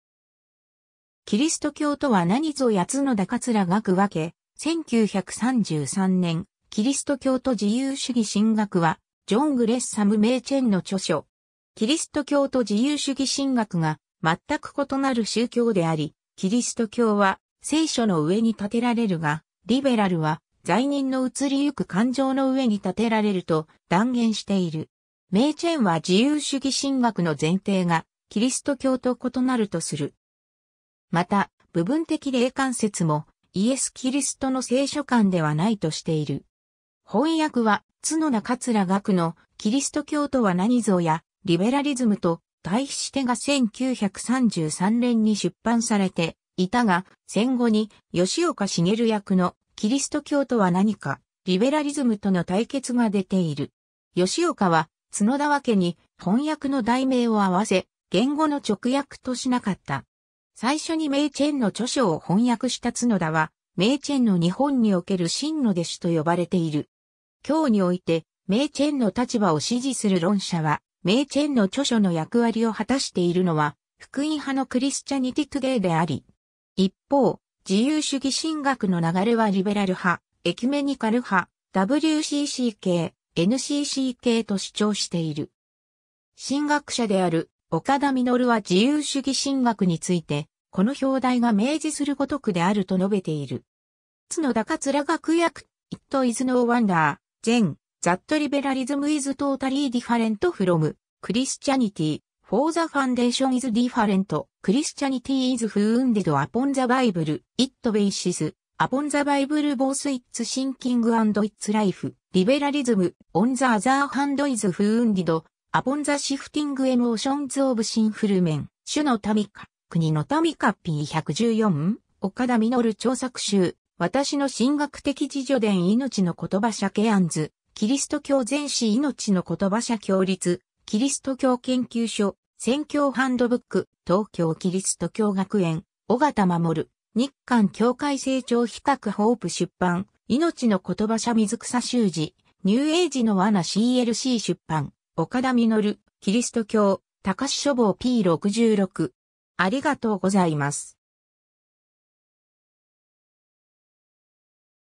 キリスト教と自由主義神学
『基督教とは何ぞや』角田桂嶽訳、1933年 キリスト教と自由主義神学（キリストきょうとじゆうしゅぎしんがく、Christianity and Liberalism）はジョン・グレッサム・メイチェンの著書。
 キリスト教と自由主義神学（リベラル）がまったく異なる宗教であり、キリスト教は聖書の上に立てられるが、リベラルは罪人の移りゆく感情の上に立てられると断言している。
 メイチェンは自由主義神学の前提がキリスト教と異なるとする。
また、部分的霊感説もイエス・キリストの聖書観ではないとしている。
 翻訳は角田桂嶽の『基督教とは何ぞや:リベラリズムと對比して』が1933年に出版されていたが、戦後に吉岡繁訳の『キリスト教とは何か:リベラリズムとの対決』が出ている。
 吉岡は角田訳に翻訳の題名を合わせ、原語の直訳としなかった。
最初にメイチェンの著書を翻訳した角田は、メイチェンの日本における真の弟子と呼ばれている。
 今日においてメイチェンの立場を支持する論者は、メイチェンの著書の役割を果たしているのは、福音派のクリスチャニティ・トゥディであり、。
一方、自由主義神学の流れはリベラル派、エキュメニカル派、WCC系、NCC系と主張している。
 神学者である岡田稔は自由主義神学について、この標題が明示する如くであると述べている。
 ^ 角田桂嶽訳 ^ It is no wonder, then, that liberalism is totally different from Christianity, for the foundation is different. Christianity is founded upon the Bible. It bases upon the Bible both its thinking and its life. Liberalism on the other hand is founded upon the shifting emotions of sinful men. ^ 『主の民か、国の民か』p.114 ^ 『岡田稔著作集』「私の神学的自叙伝」いのちのことば社 ^ ケアンズ『基督教全史』いのちのことば社 ^ 共立基督教研究所『宣教ハンドブック』東京キリスト教学園 ^ 尾形守『日韓教会成長比較』ホープ出版、いのちのことば社 ^ 水草修治『ニューエイジの罠』CLC出版 ^ 岡田稔『基督教』高志書房 p.66